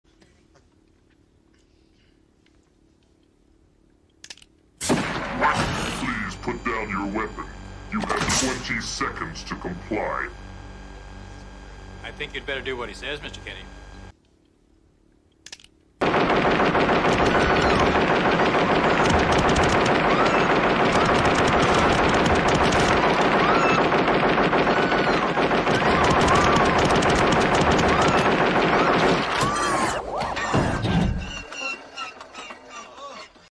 Kendrick vs Drake beef explained in 34 seconds
Basically.